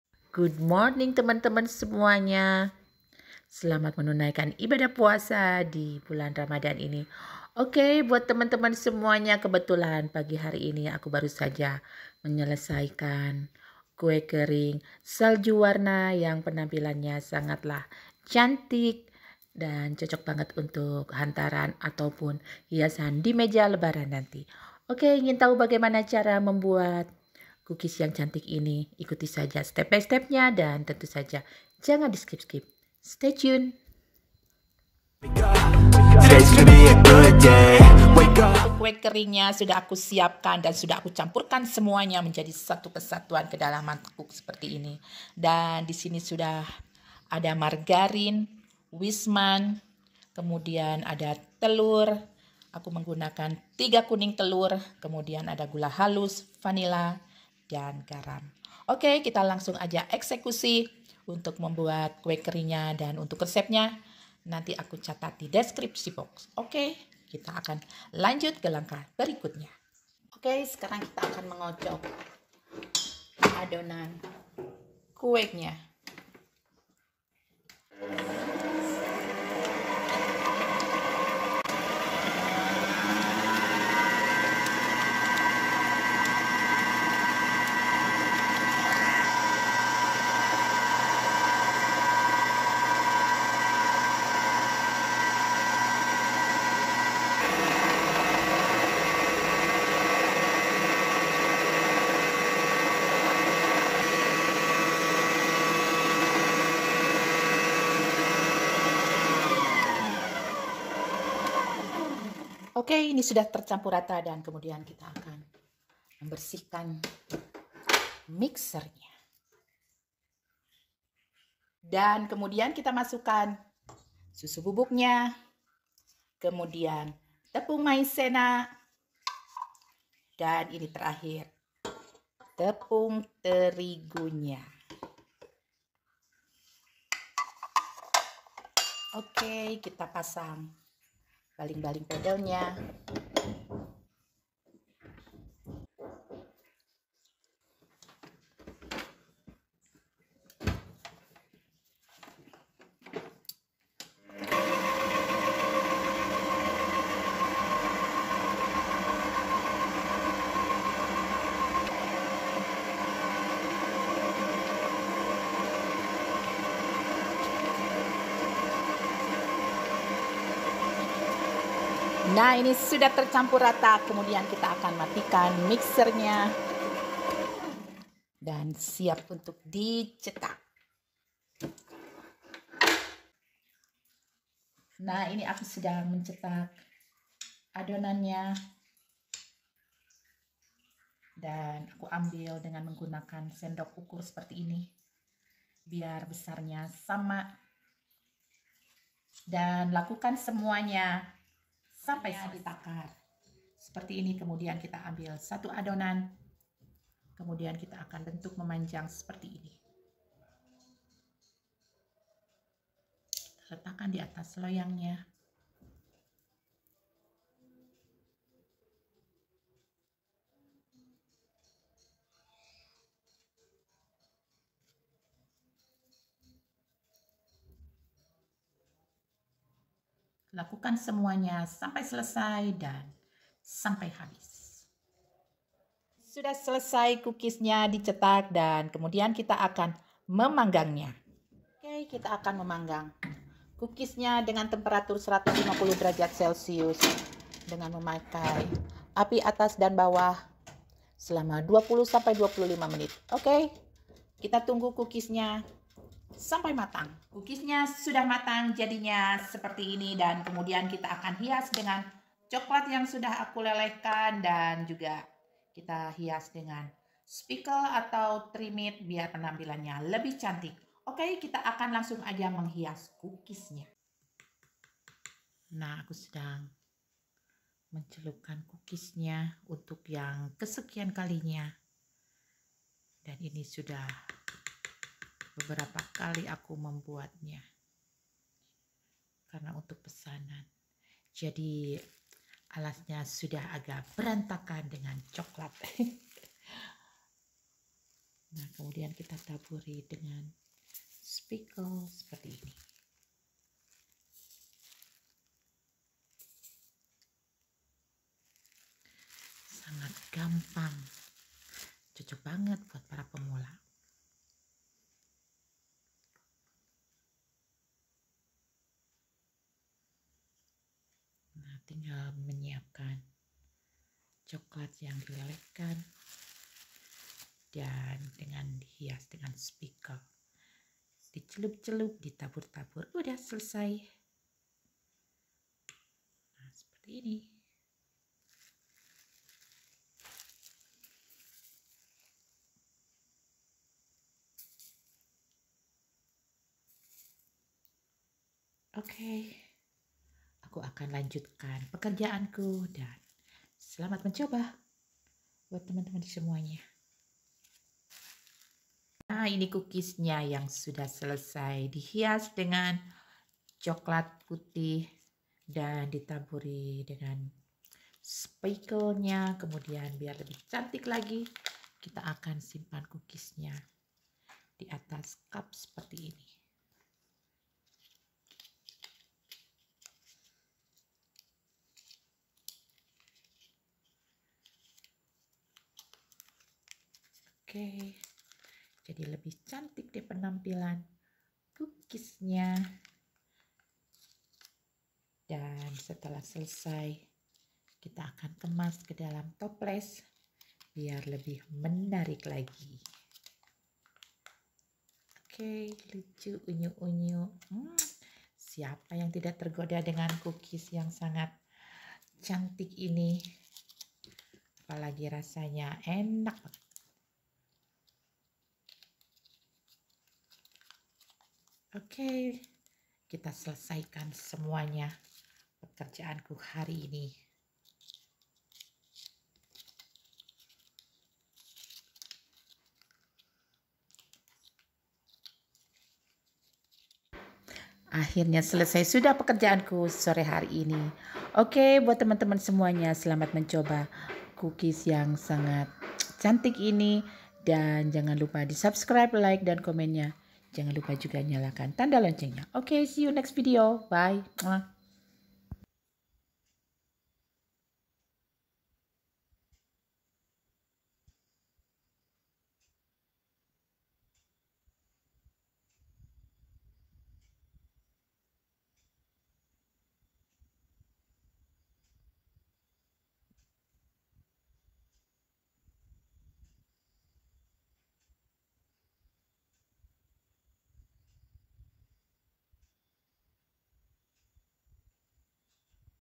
KUE KERING SALJU WARNA SELALU JADI REBUTAN ANAK ANAK DISAAT HARI RAYA
BAHAN :

150    GRAM     MARGARIN
100    GRAM     WISMAN /CORMAN
2         BUTIR     KUNING TELUR
100     GRAM     SUSU  BUBUK
130     GRAM     GULA  HALUS
450     GRAM     TEPUNG SERBAGUNA
1          SDT         GARAM
1          SDT         VANILLA

TOPPING  :

300   GRAM    WHITE CHOCOLATE
100    GRAM   TRMIT SECUKUPNYA

              🍪🍩SELAMAT MENCOBA 🍩🍪

         🔔🔔SUBCRAIBE  LIKE COMENT🔔🔔